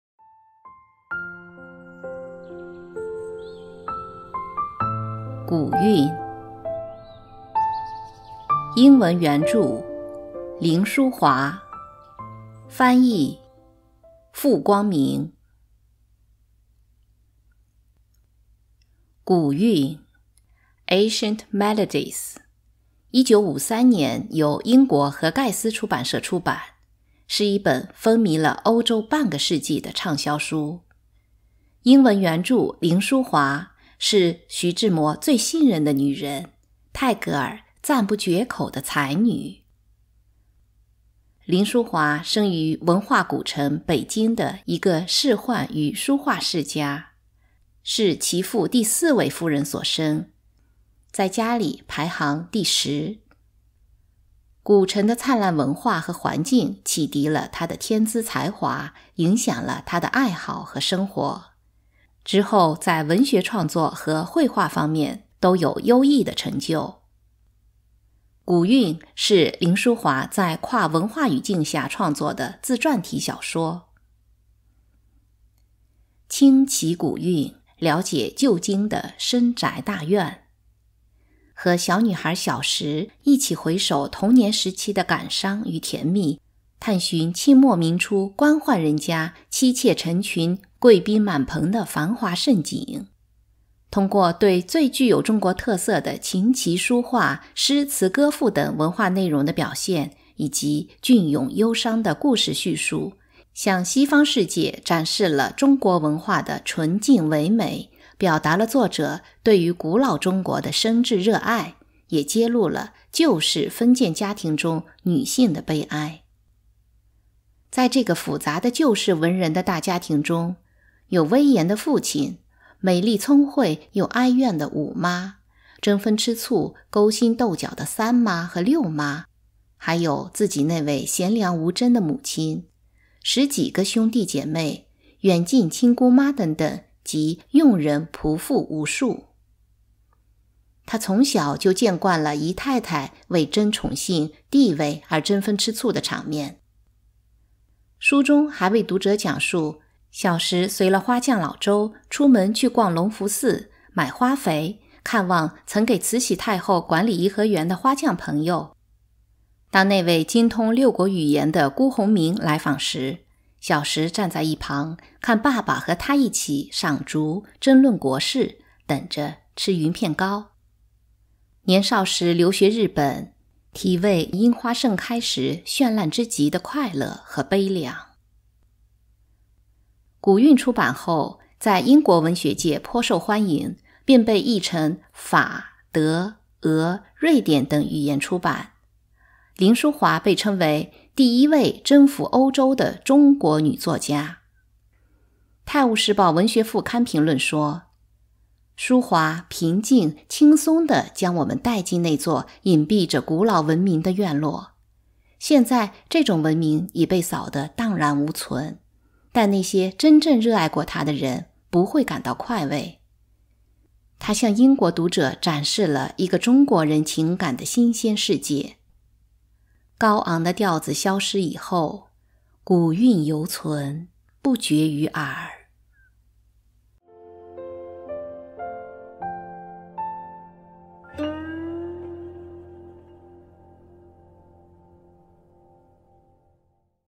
簡介《古韻》（民國才女凌叔華 風靡了歐洲半個世紀的小說）
《古韵》Ancient Melodies是民国才女凌叔华在跨文化语境下创作的自传体小说，1953年由英国荷盖斯出版社出版，风靡了欧洲半个世纪。
"高昂的调子消失以后，古韵犹存，不绝于耳。”
环佩叮当演播
Odysee 环佩叮当